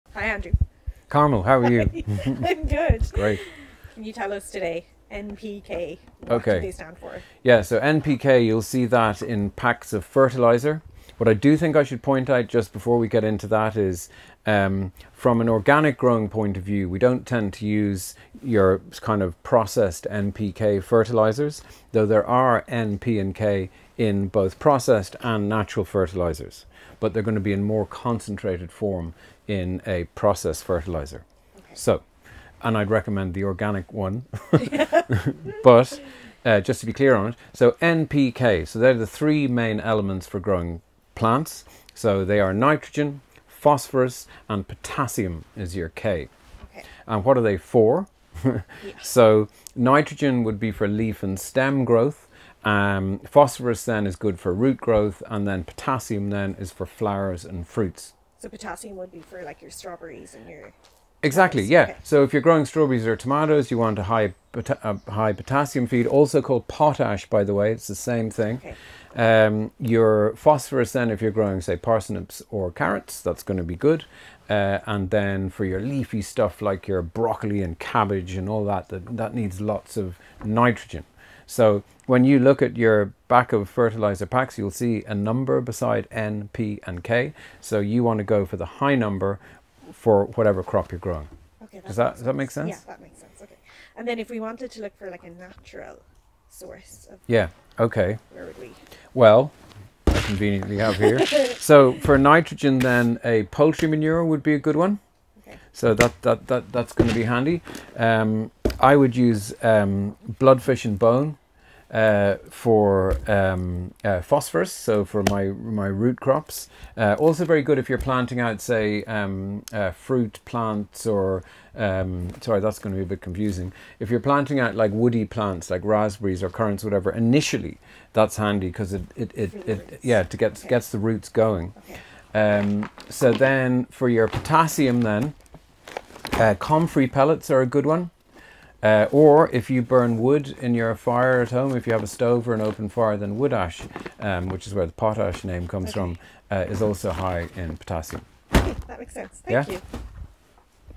Plant Feed - NPK Explained
NPK - A Quick Guide.

Hope you're enjoying the season 🙂

If you are new to growing, plant feeds can be confusing but once you learn how to read and understand NPK numbers on packs it will make choosing the right one a lot easier.

N=Nitrogen
P=Phosphorous
K=Potassium

🍅 Plants need nitrogen for leaf growth, phosphorus for root growth, and potassium for fruit growth. All plants need all three, but leafy crops particularly need nitrogen, and fruit crops won't develop well without enough potassium. When you look at a bag of fertilizer you will see the NPK values displayed as 3 numbers.

🍅 Let's use an example of organic strawberry feed. The NPK ratios are displayed as 4:2:6 meaning the feed contains 4 nitrogen for foliage, 2 phosphorous (P) for roots and a higher 6 potassium (K) to support the strawberry fruit. It is higher in potassium because it is a fruiting plant feed. A tomato plant feed will have a similar ratio meaning a tomato feed will work just as well for strawberries and vice versa; they are just specifically labelled to assist you in buying more fertiliser...

🍅 Cabbage, broccoli, chard and other leafy crops, on the other hand, will benefit from a higher nitrogen feed because nitrogen is needed for green, leafy growth. Feeds high in nitrogen should be avoided on fruiting plants because they will grow lots of leaf at the expense of the fruit.

🍅 When looking for the correct feed ask yourself what part of the plant are you interested in? The leaf (spinach), the root (carrot) or the fruit (tomato)? Once you know nitrogen is for leaf growth, phosphorus is for roots and potassium for is fruit you can easily make the right choice.

🍅 The more you keep building up your soil with mulches every year, the less fertiliser/plant feed you will need to add. Think seaweed, well rotted manure, homemade garden compost etc.